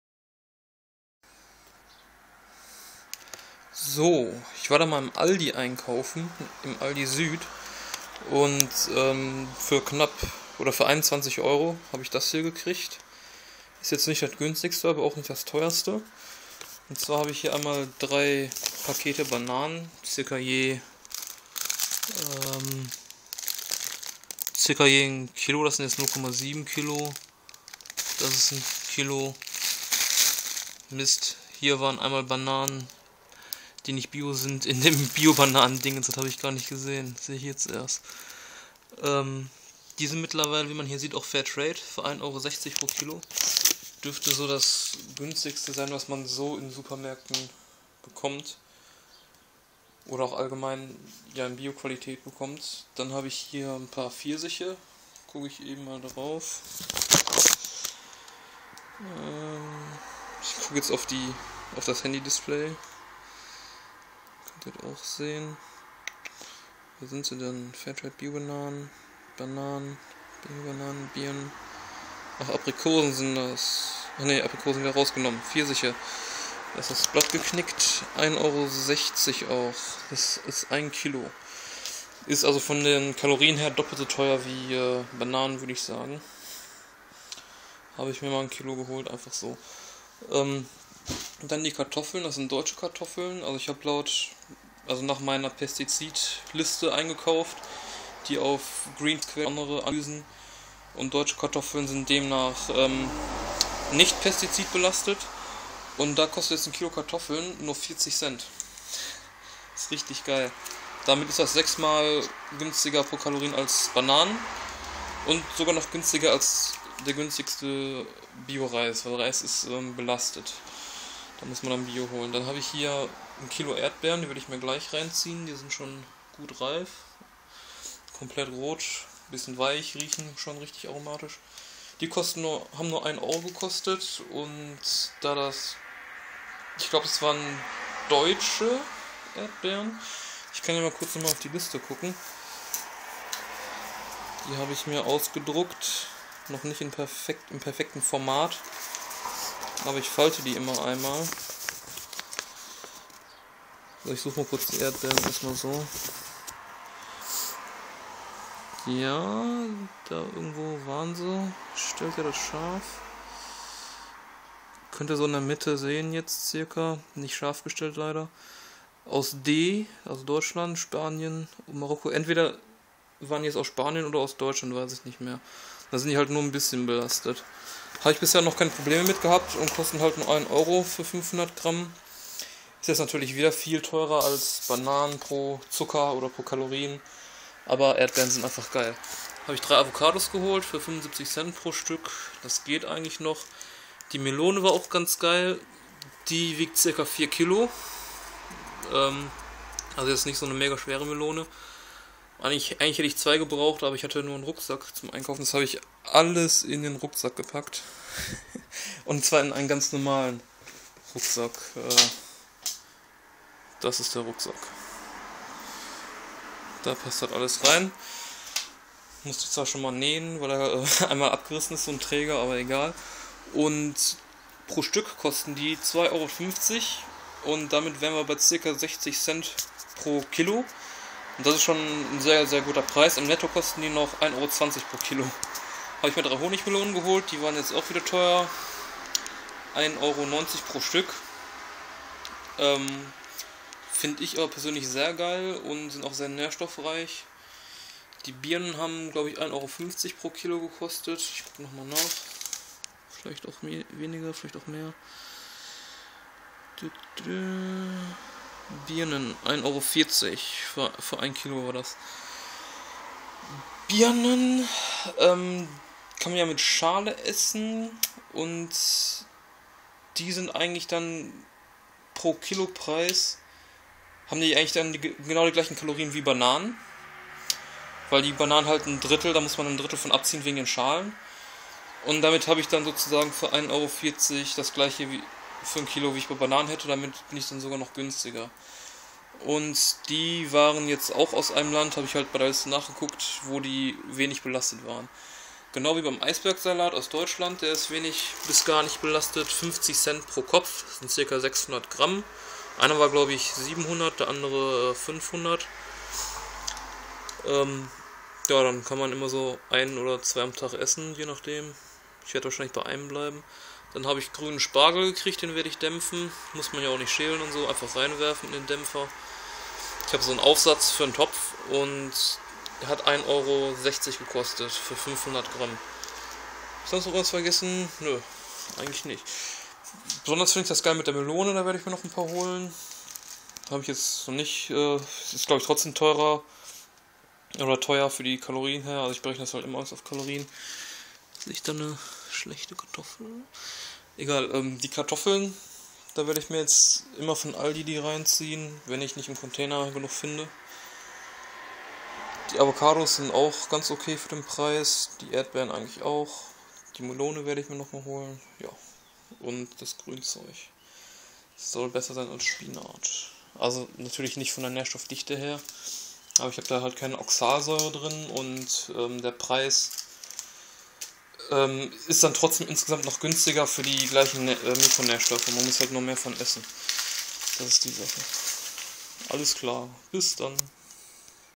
Carbivore Aldi Haul ^^
Bananen-Kiste war noch nicht reif, für die nächsten 2-3tage daher nen Aldi Süd Einkauf:

3x avocado 75c
3x stauden(?) bananen bio fairtrade 1,6€/kg
2,5kg kartoffeln ~40c/kg
1kg erdbeeren 2€
3x honigmelone 1,9€ :/
1kg eisbergsalat 1€
4kg wassermelone 2,5€/stk.
1kg birnen 1,4€ (oder warens 500g? packung hab ich schon beim aldi weggeschmissen)
1kg pfirsiche 1,6€
500g grünen spargel 1,6€

alles niedrig o. gar nicht belastet laut greenpaece. Link zur kompletten Liste samt Quellen findet ihr in einen der letzten Videos!